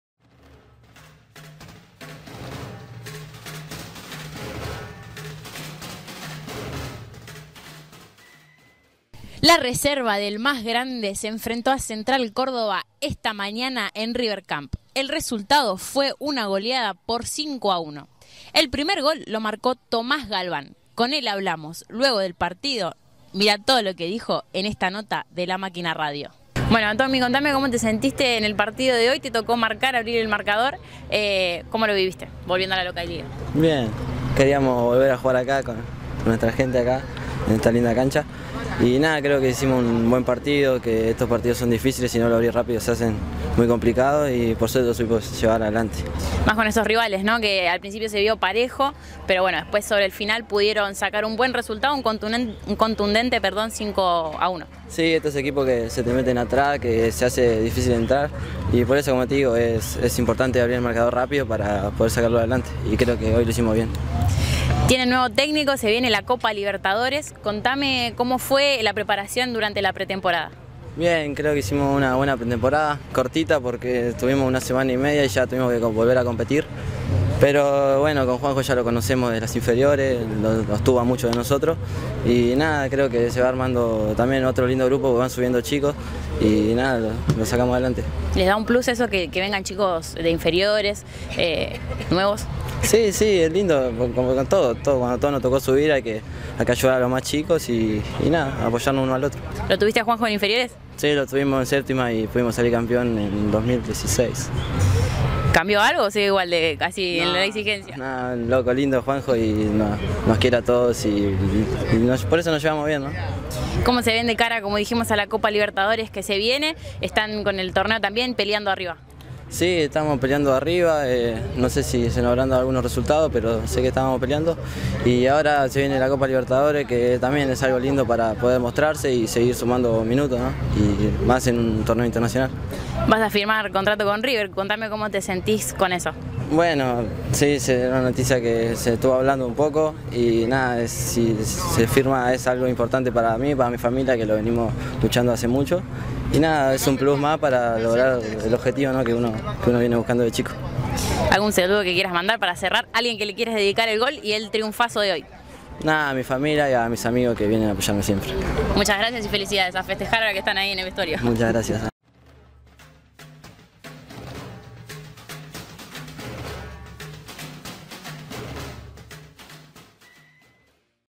TOMÁS GALVÁN: "A JUANJO LO TUVIMOS EN INFERIORES, NOS CONOCE A TODOS"
Mano a mano con Tomás Galván, autor del primer gol de la Reserva de River en la victoria ante Central Córdoba de Santiago del Estero por 5 a 1.

River Camp - 31/1/2020

Nota: Andy Gómez
Cámara: Ubaldo Kunz